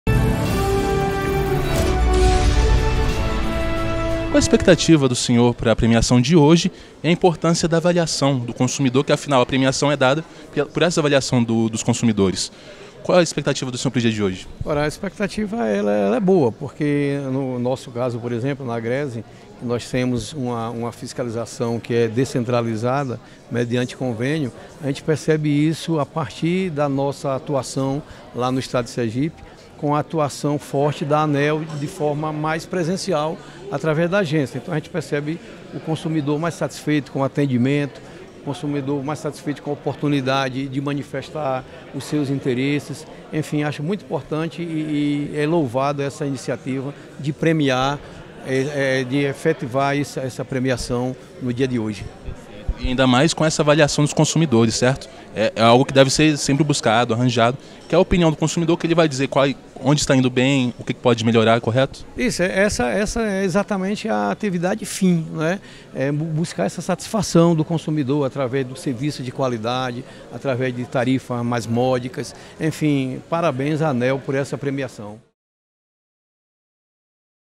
ESTADOS A satisfação dos consumidores de energia para as agências estaduais conveniadas
Para Luiz Hamilton Santana de Oliveira, diretor presidente da Agência Reguladora de Serviços Públicos do Estado de Sergipe (Agrese), a pesquisa anual com os consumidores de todo o País contribui para o trabalho das agências reguladoras estaduais com as quais a ANEEL possui parceria.

Você sabia? O Índice ANEEL de Satisfação do Consumidor (Iasc) é um indicador que permite avaliar a satisfação do consumidor residencial com os serviços prestados pelas distribuidoras de energia elétrica. O Iasc é obtido anualmente a partir de pesquisa amostral realizada com consumidores de todas as distribuidoras, concessionárias e permissionárias, que atuam no território nacional.